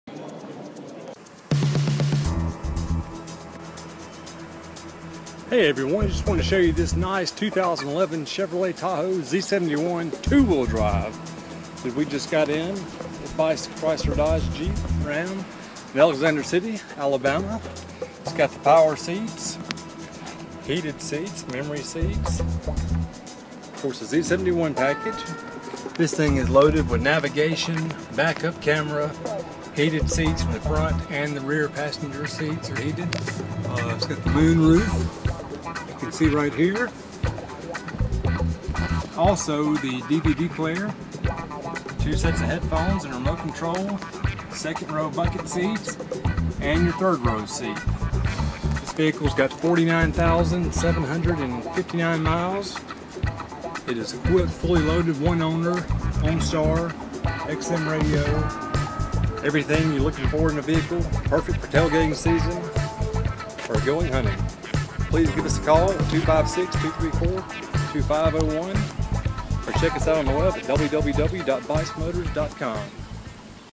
Used 2011 Chevy Tahoe Z71 nav DVD backup camera leather alexander city al
2011 Chevy Z71 2wd Tahoe.  Leather, Heated Seats, Nav, Sunroof/moonroof, captain chairs, 3rd row seats, DVD Player, Backup Camera. Bice Chrysler Dodge Jeep Ram
2133 Cherokee Rd Alexander City, AL 35010